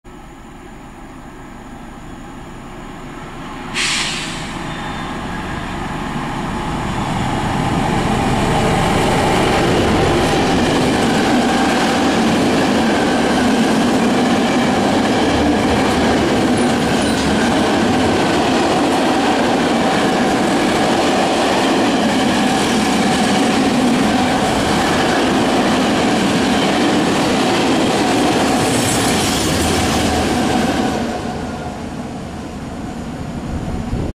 SM42-521 PKP Cargo ze Składem towarowym
Przejazd stonki w nocnym klimacie ze składem węglarek w kierunku Ciechanowa.Pozdrowienia dla towarzysza Tomka.
Stupsk 3 Listopada 2014 rok . Godzina 20:11